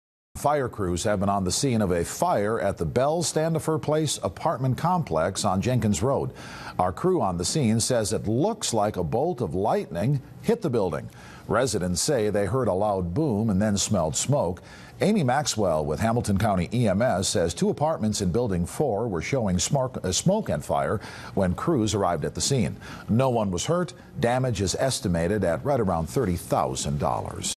Lightning Strike Causes Apt. Fire
Fire crews are on the scene of a fire at the Standifer Place Apartment Complex. Our crews on the scene says a bolt of lightning struck building 4 of the complex and caused a significant amount of damage.Residents say they heard a loud boom and then smelled smoke.No word on any injuries or the extent of damage, but depend on us to provide that information when it becomes available.By Natalie Jenereski